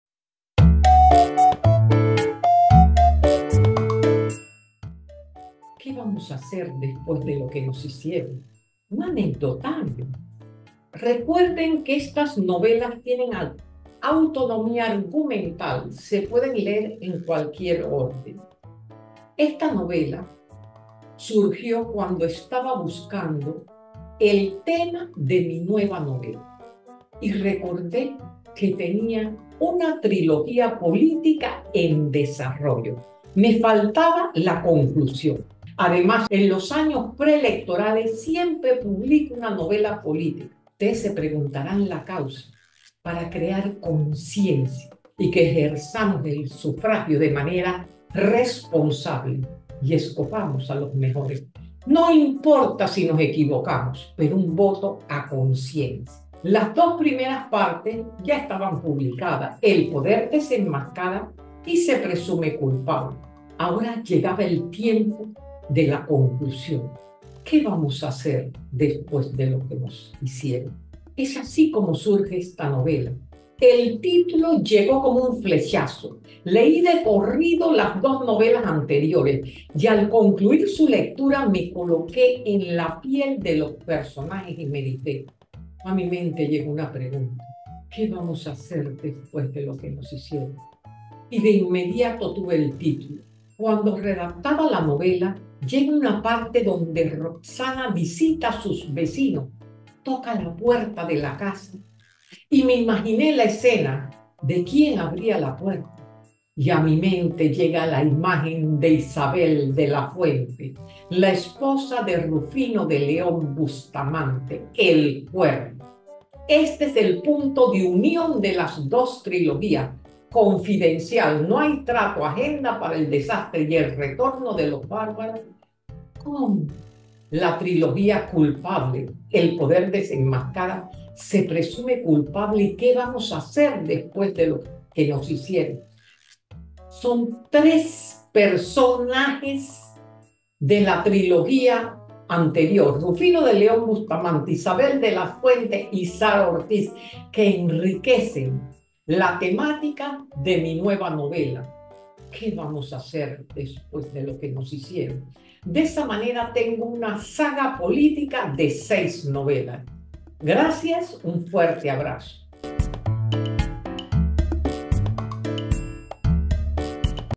ANECDOTARIO ¿QUÉ VAMOS A HACER DESPUÉS DE LO QUE NOS HICIERON?
La respuesta que demos a tal interrogante dependerá el destino de los actores, o de nosotros mismo, una vez que, sintiéndonos aludidos y nos aboquemos a contestarla. Es cierto que múltiples serán las respuestas, como múltiples las personas que han sido abusa por un sistema judicial corrupto. Que siente un inocente que no solo se presume culpable, sino que se le condena.
Esta es una obra de ficción, la novela siempre lo es, aunque narre eventos reales. No obstante, este no es el caso. Una novela como esta, es en cierta medida impersonal. Responde a una toma de conciencia concebida por todos, alguien la ejecuta y la novelista se eclipsa, porque es solo narradora del pensamiento, de la reflexión, del sentir general de una buena parte de los ciudadanos. Cuando la ficción se fusiona con la realidad y con nuestros temores, se produce una obra que nos obliga a procurar un mejor país, decente y democrático, donde se respeten los derechos de los ciudadanos, en la que se les presuma inocentes hasta que se compruebe su culpabilidad.